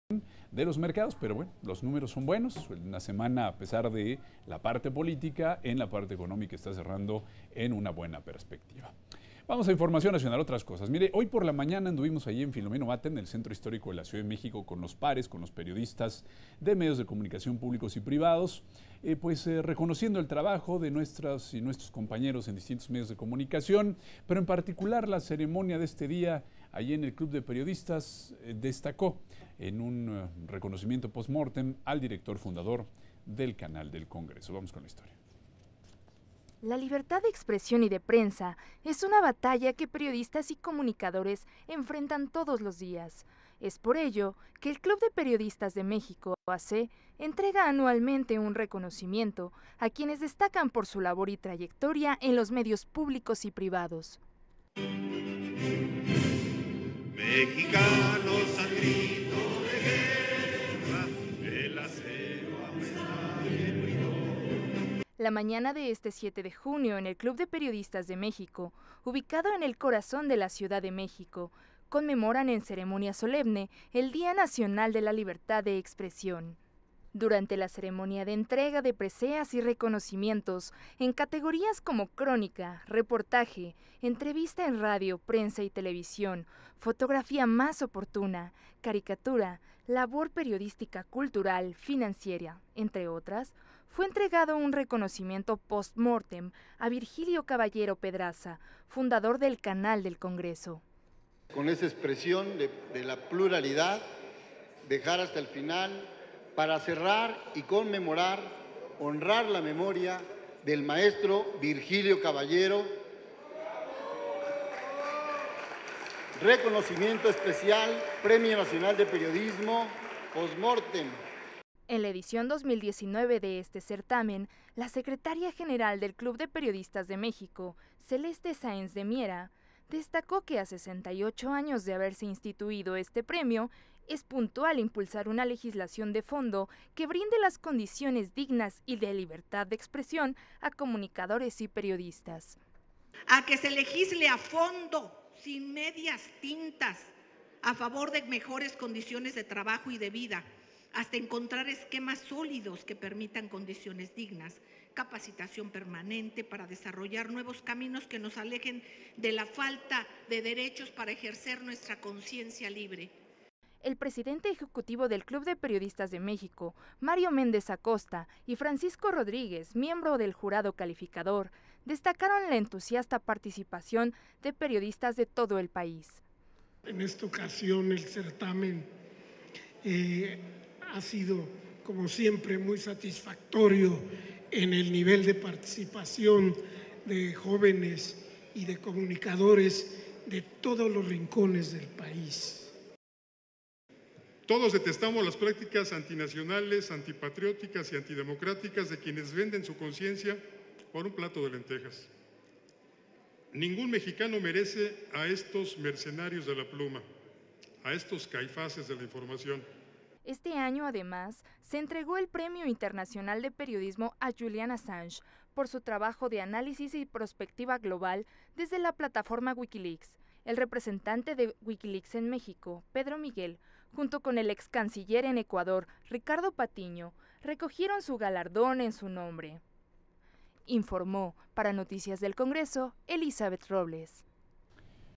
Premio Nacional de Periodismo 2019
La libertad de expresión y de prensa es una batalla de periodistas y comunicadores enfrentan todos los días, Por ello, Club de Periodistas de México A.C, entrega anualmente un reconocimiento a quienes destacan por su labor y trayectoria en los medios públicos y privados.